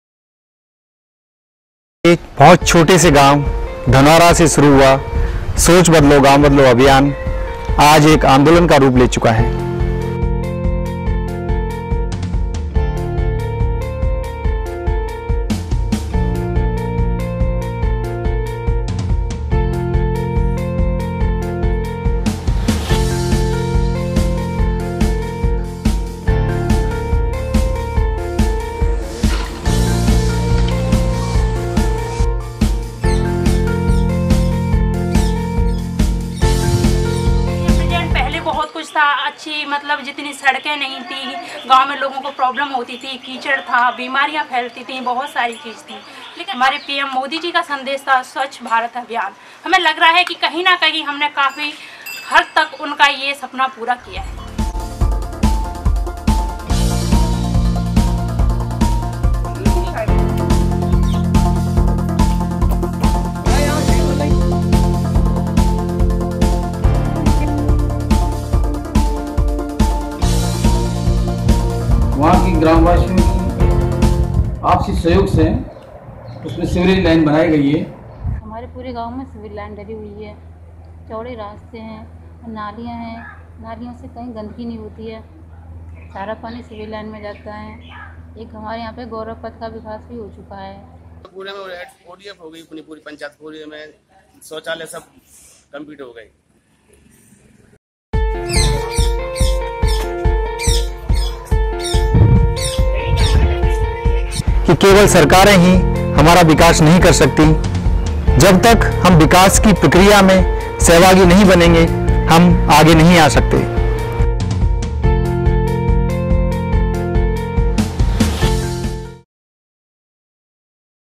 धौलपुर के इस गांव को आदर्श ग्राम कैटेगरी में मिला पहला स्थान, पीएम ने किया स्म्मानित
धौलपुर। धनोरा गांव भारत में संस्थागत विकास का मॉडल बनकर उभरा है। स्मार्ट विलेज धनोरा को आदर्श ग्राम सम्मान कैटेगरी में पहला स्थान मिला है।  सोच बदलो-गांव बदलो मुहिम से गांव का विकास हुआ है। उप राष्ट्रपति और प्रधानमंत्री सोमवार को दिल्ली में सम्मानित करेंगे। इस गांव को देख हर कोई चकमा खा जाता है कि वाकई ये कोई गांव है या आधुनिक शहर। यह किसी अन्य सामान्य गांव से बिल्कुल जुदा है।